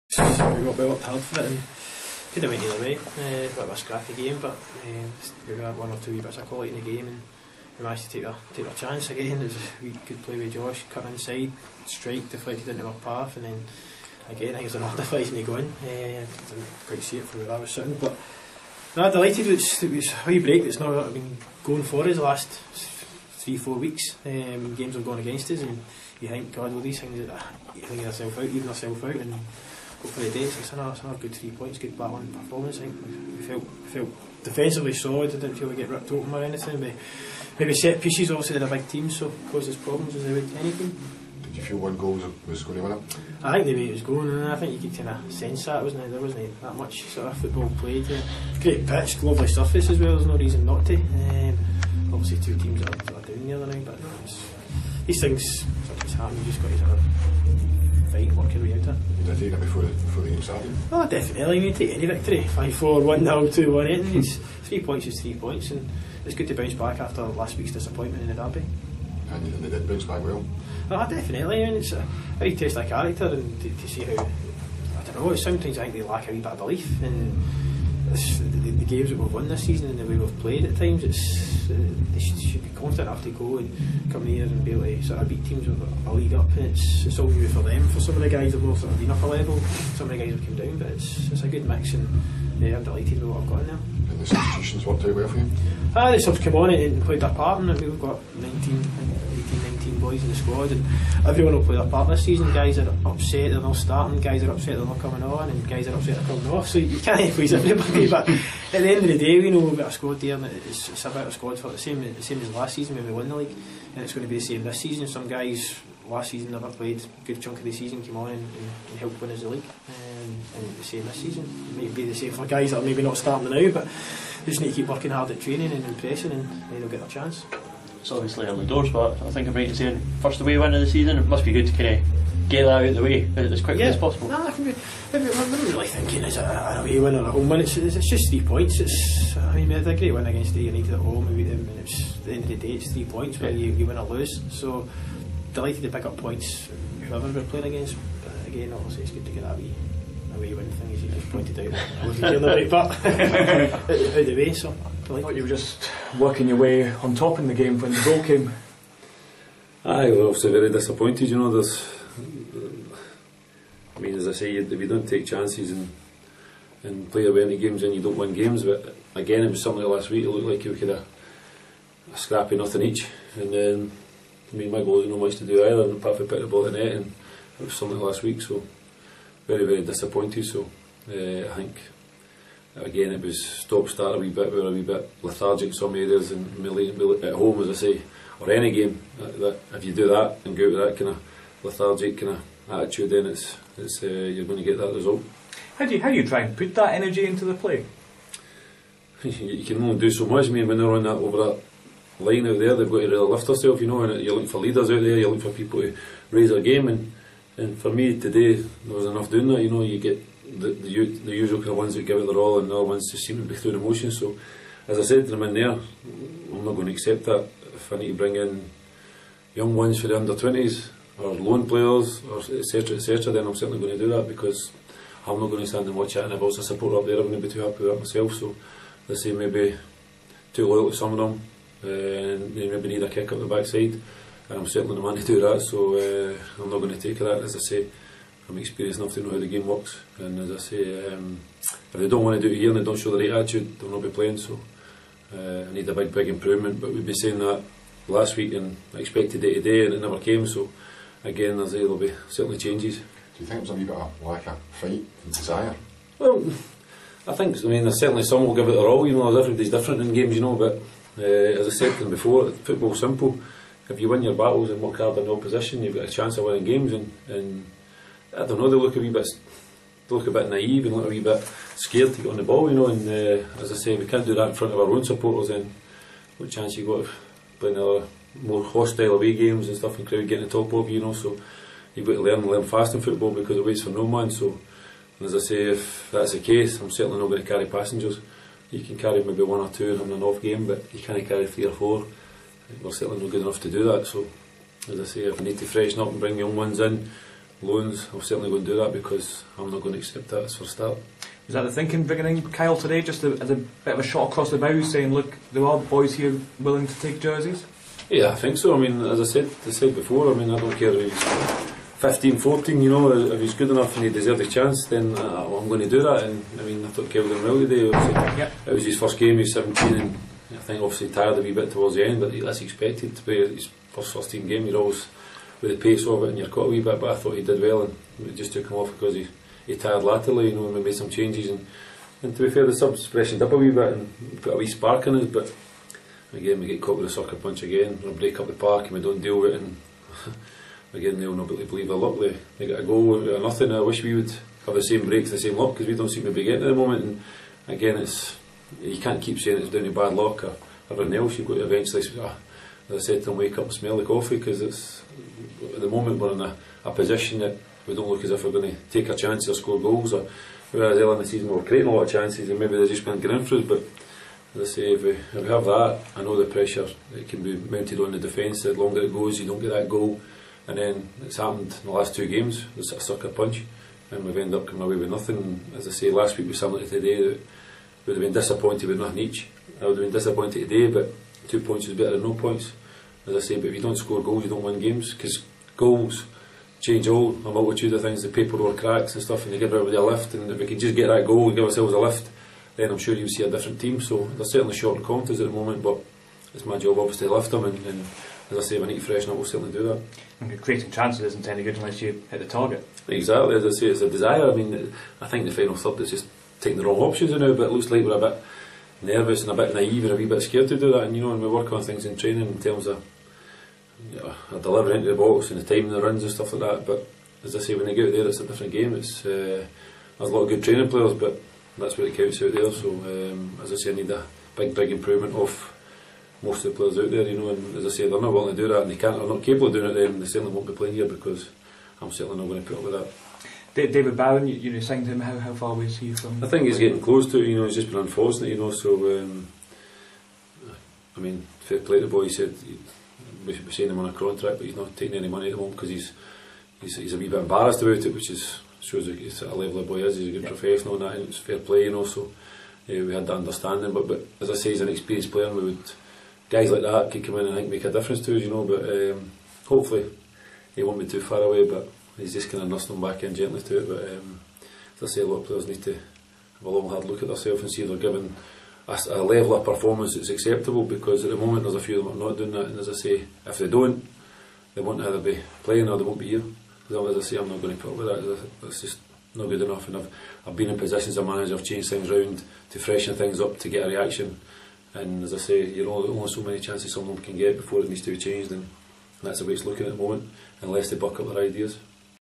Albion Rovers and Stranraer Manager Interviews 190915
Darren Young and Brian Reid give their views after Albion Rovers beat Stranraer at Stair Park.